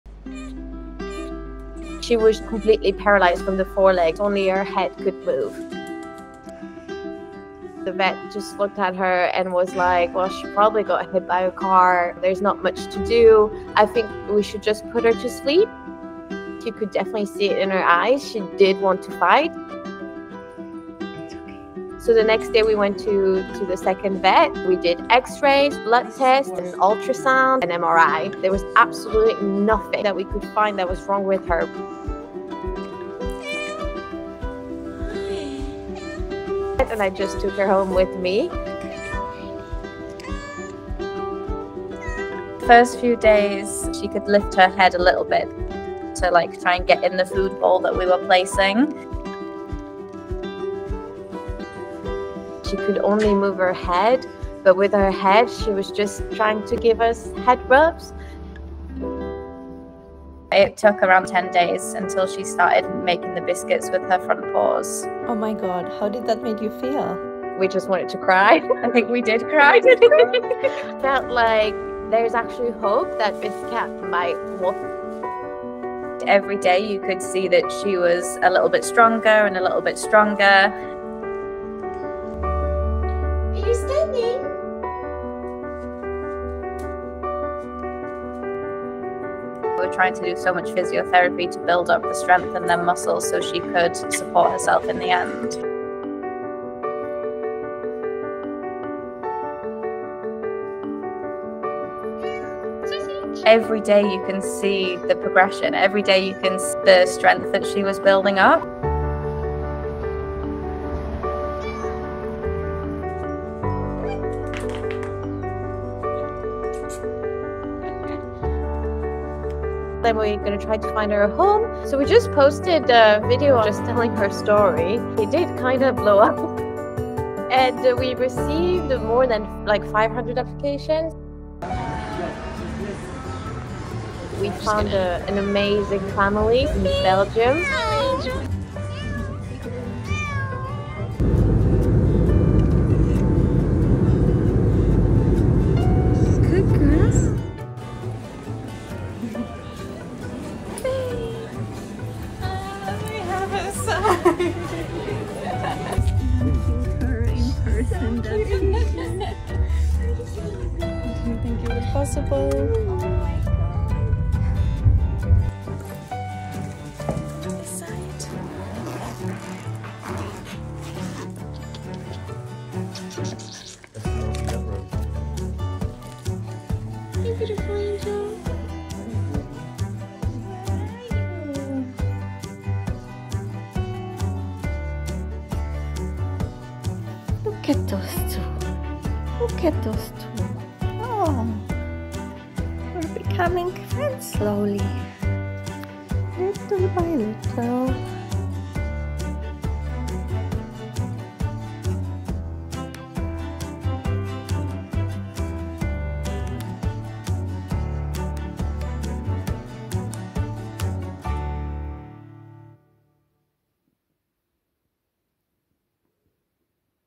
Watch This Stray Cat Who Couldn't Move Defy All The Odds | The Dodo
No one knew why this cat could only move her head — wait to see her surprise everyone 💜

Keep up with Saaving Lives' rescue efforts by following them on Instagram TikTok & Facebook